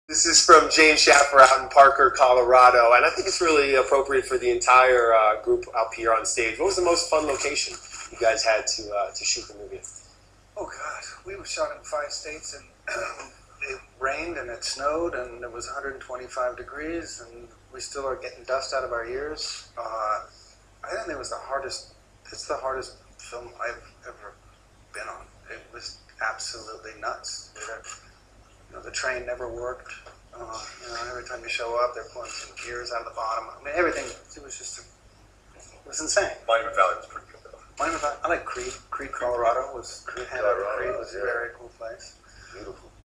Johnny Depp Creede, Colorado
Johnny Depp and Lone Ranger Director, Gore Verbinski mention Creede, Colorado as one of their favorite places that they filmed in.